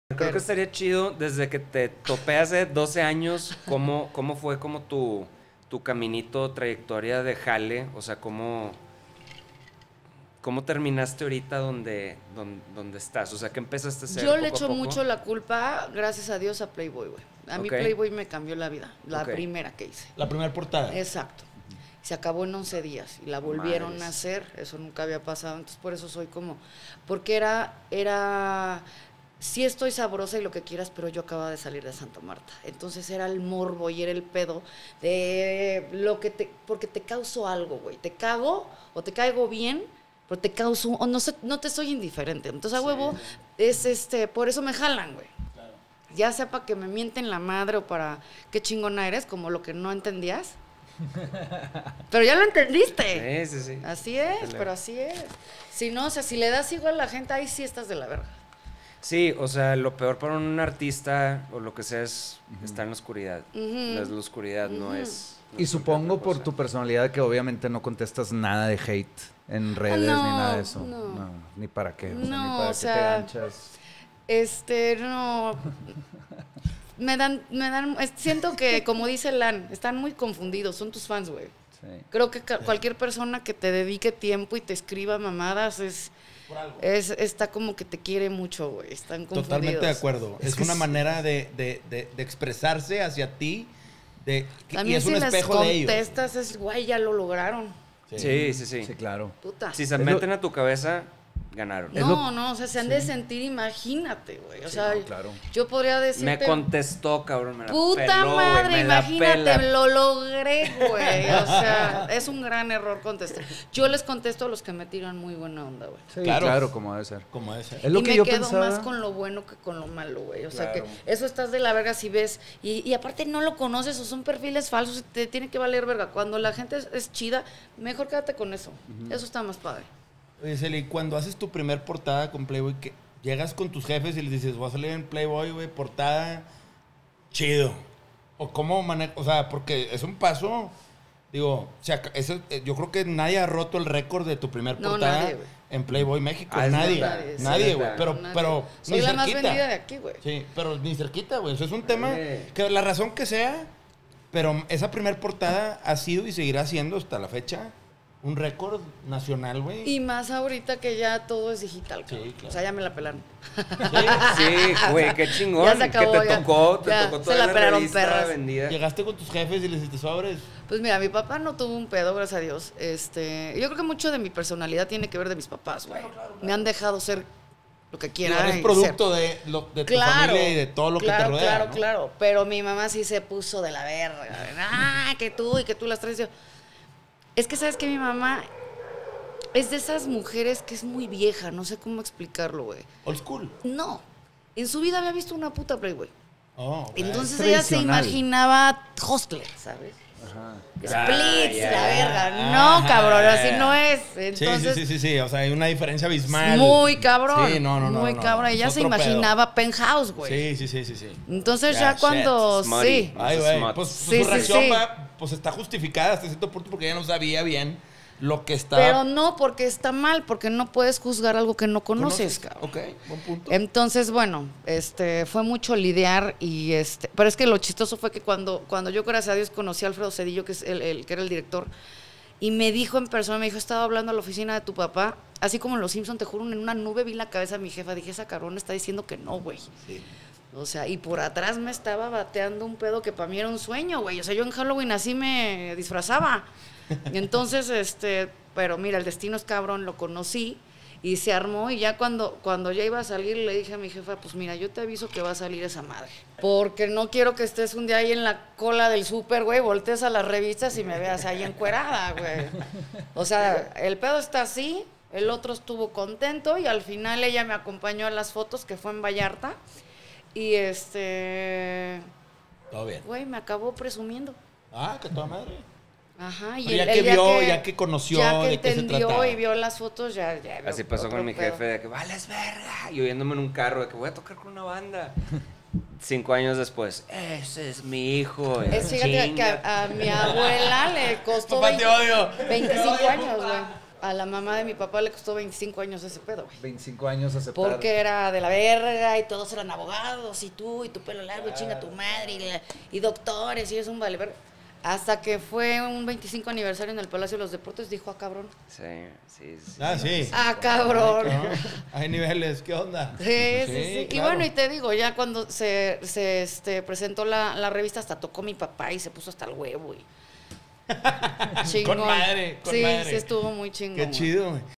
Celia Lora: "Playboy me cambió la vida"
En este nuevo episodio nos fuimos a CDMX para platicar con Celia Lora. No puedes perderte esta gran platica en donde nos platican de sus inicios como modelo en Playboy México, como ha evolucionado su contenido en las diferentes plataformas y como ha manejado la fama y los comentarios mas locos de gente en sus redes sociales.

Episodio 014 de SELLOUT by Negro Pasión.